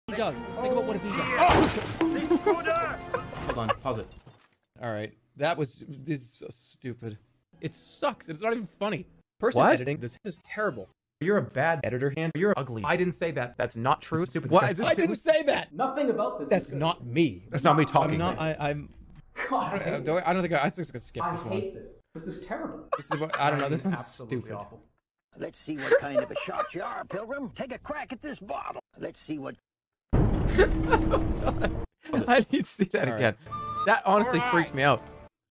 Jerma harshly critizises greenscreen edit
Jerma reacts to him harshly critizising a greenscreen edit.